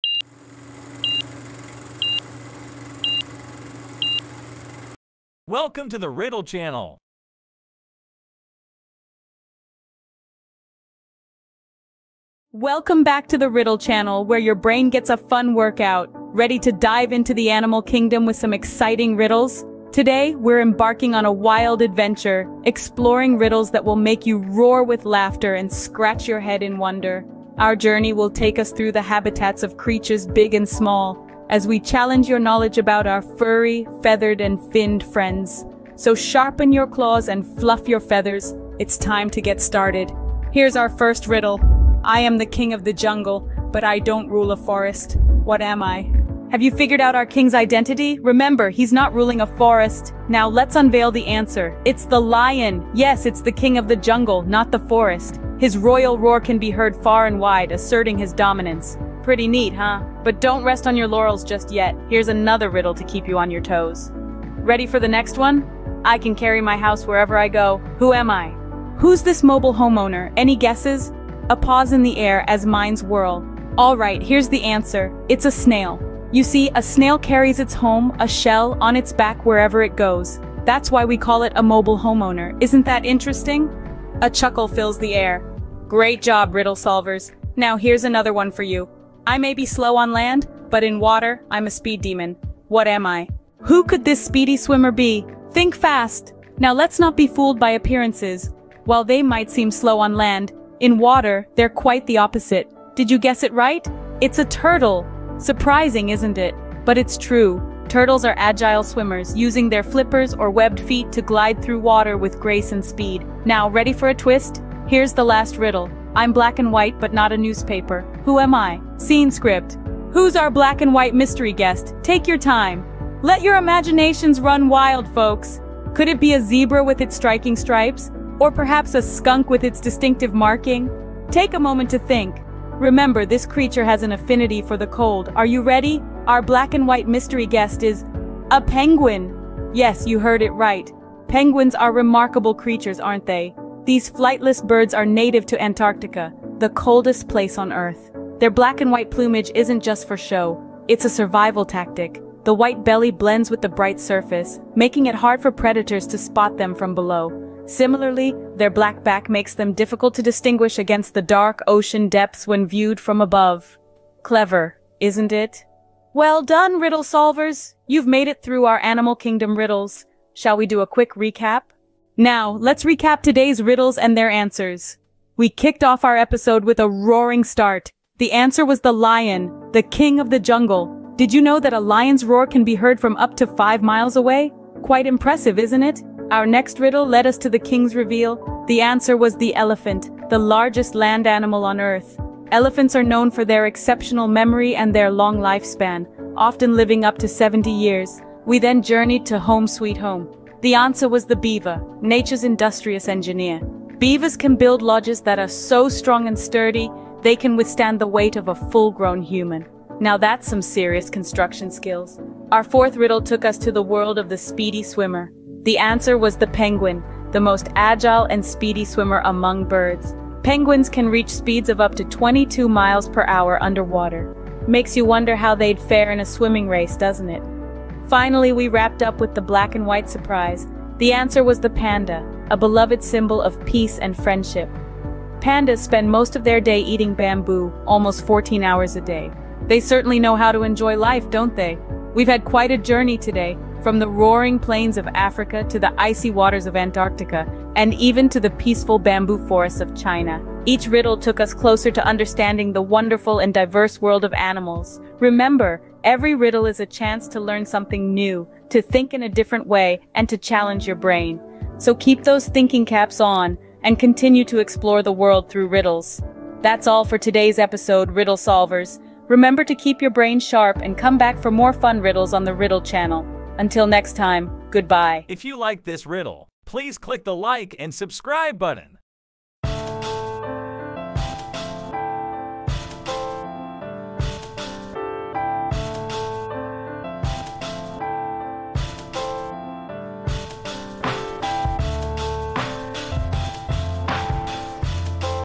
Funny Animal Riddles Brain Teasers for the Wild at Heart
Unleash your inner beast with "Funny Animal Riddles: Brain Teasers for the Wild at Heart" – a wildly entertaining collection that promises to tickle your funny bone and challenge your wits! Perfect for the animal lover with a penchant for puzzles, this book is packed with a menagerie of clever riddles, each featuring our furry, feathered, and finned friends from the animal kingdom.

Are you ready to prowl through puzzles, soar through wordplay, and leap into lateral thinking? These riddles range from easy peasy 'kitten' questions to 'bear' bonkers brain-teasers that will have you scratching your head as if you've got fleas!

Here's what to expect:
- A critter-filled compilation of riddles, each with a comical twist that ensures giggles along with the head-scratching.
- An array of challenges that will test your knowledge of animal habits, habitats, and hilarities.
- 'Paws’-itively perfect puns to make learning fun and engaging for all ages.
- An interactive experience with opportunities to guess, laugh, and even learn some wild animal facts.

Whether you're a spirited sprout or a wise old owl, "Funny Animal Riddles: Brain Teasers for the Wild at Heart" is the perfect book to share with family and friends, or to enjoy solo for a safari of smirks and smarts. So, fluff up your feathers, sharpen your claws, and get ready to dive into a jungle of jovial jests and japes!

Grab your copy and let the wild rumpus of riddles begin!